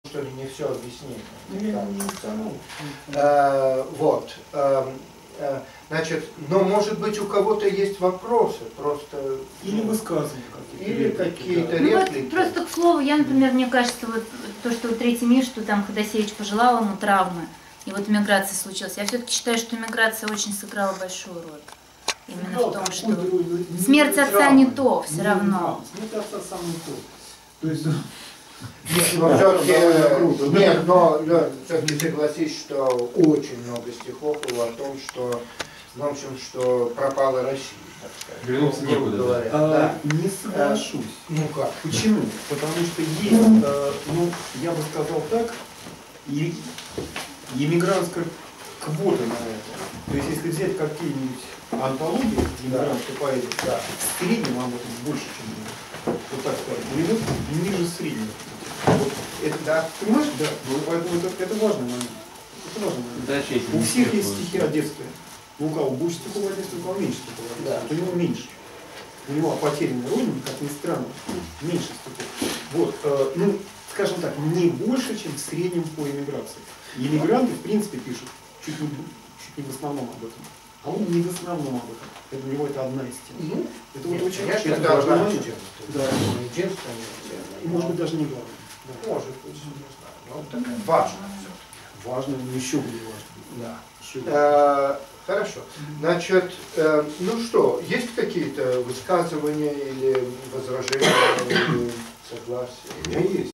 28 09 13 (6)
Третья встреча цикла "Человек в других людях" Тема - "Георгий Иванов глазами человека моего поколения". Главный докладчик-рассказчик - литератор Леонид Костюков. Содокладчик - поэт Андрей Гришаев. Ведущий - Дмитрий Веденяпин, поэт, переводчик.